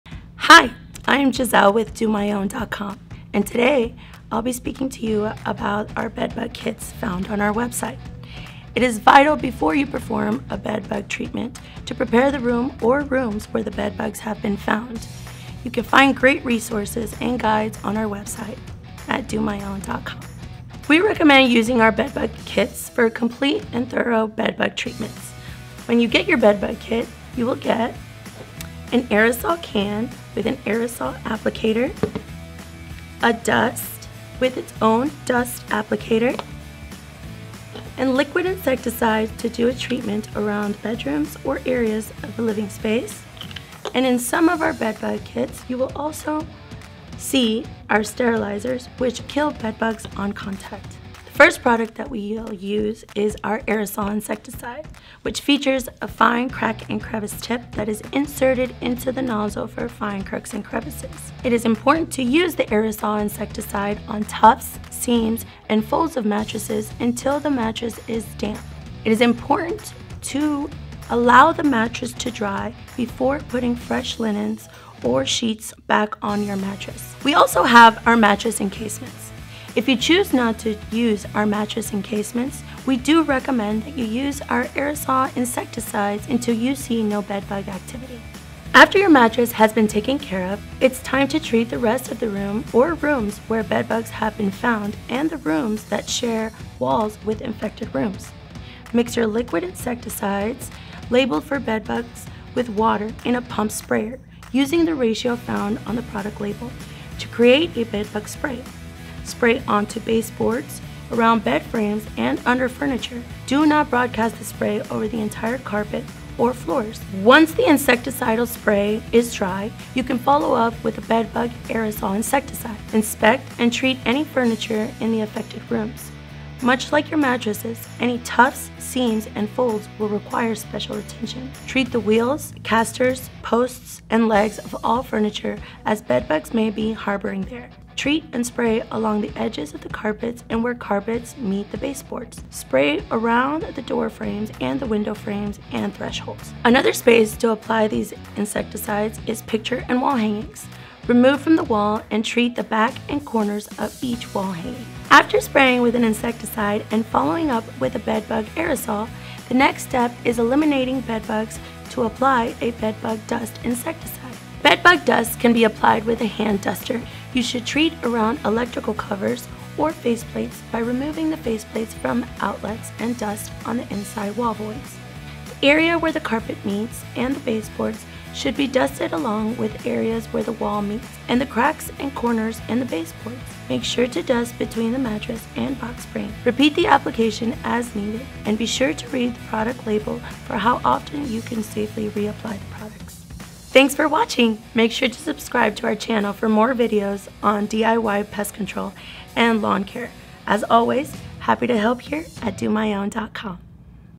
DoMyOwn.com Bed Bug Kits
The DoMyOwn bed bug kits provide you with the professional products necessary to do a complete bed bug treatment in your home or apartment.

Our bed bug kits 1-3 contain concentrated insecticide, insecticide dust for bed bugs, hand dusting tool, bed bug aerosol spray, our exclusive step-by-step guide. Bed Bug Kit 2 and 3 contain a sterilizer that kills bed bugs on contact.

NOTE: Before you begin your own bed bug treatment, you will need to prepare the room or rooms where bed bugs have been found, in addition to rooms that share walls with the infested rooms.

Always make sure to read the product label of any product you're using before applying it. We also provide free expert advice and are available for any questions you may have on product recommendations or application instructions.

-timestamps-
0:11 Prepare room(s) where you are treating
0:26 What comes with my bed bug kit?
0:51 Bed Bug Mattress Treatment with Aerosol
1:18 Mattress Encasements
1:36 Liquid Insecticide Treatment
2:02 Insecticide Aerosol Treatment